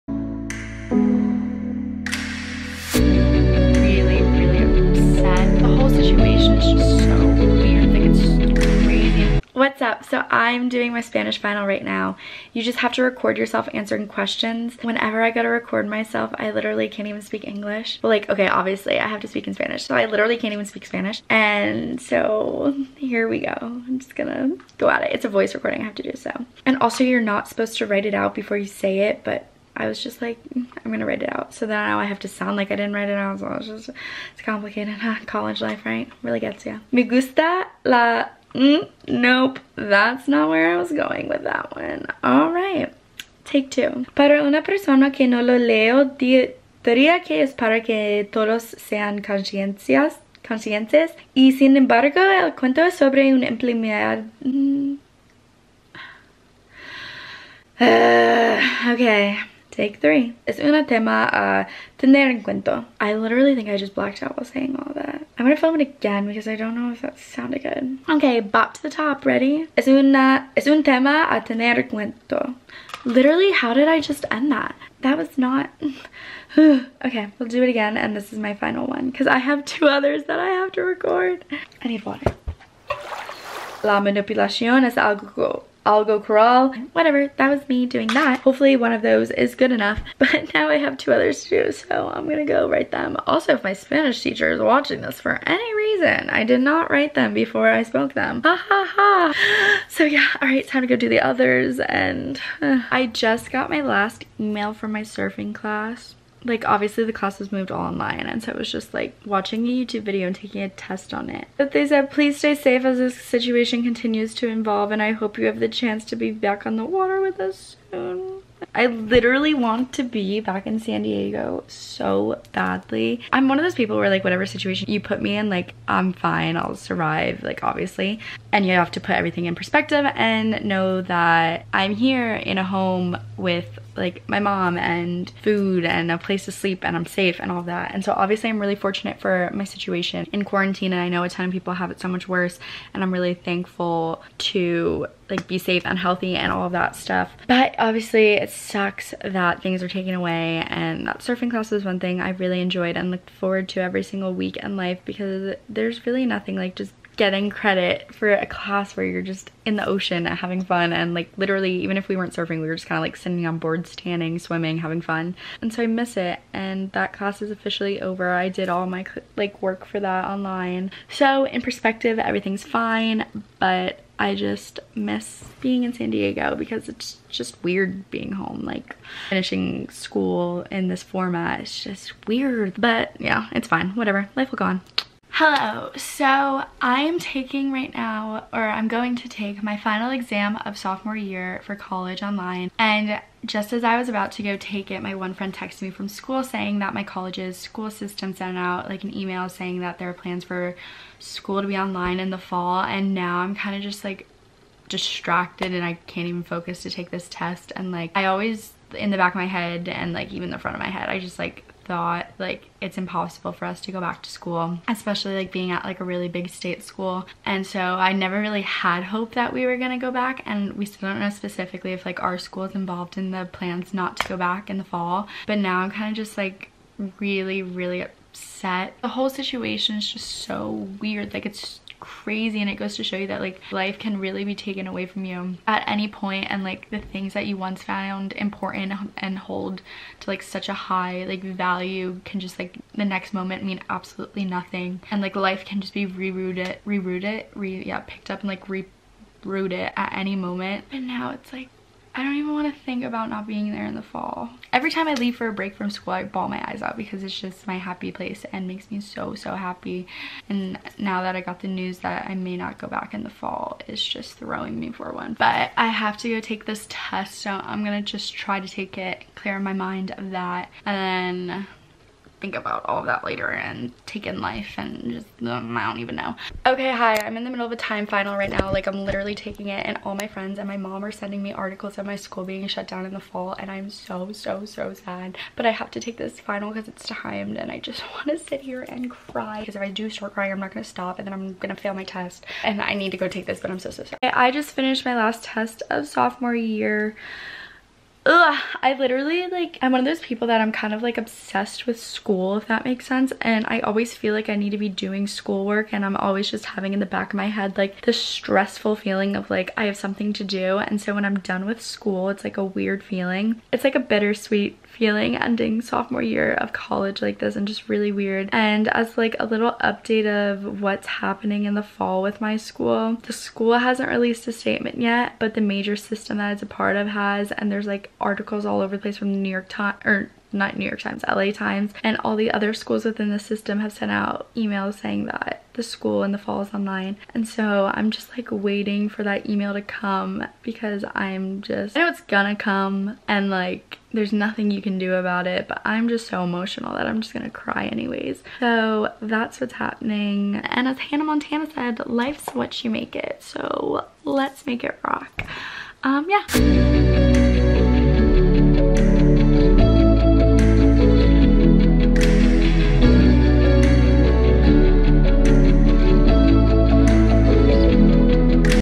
college finals week + CANCELLED FALL SEMESTER!
WELCOME TO A FINALS WEEK IN MY LIFE AT San Diego STATE UNIVERSITY!!! + the news of the fall semester :(

I filmed this a few months back... literally 5 months ago lol whoops but since it's ~emotional~ & ~sad memories~ I just got around to editing it so I hope you enjoy :)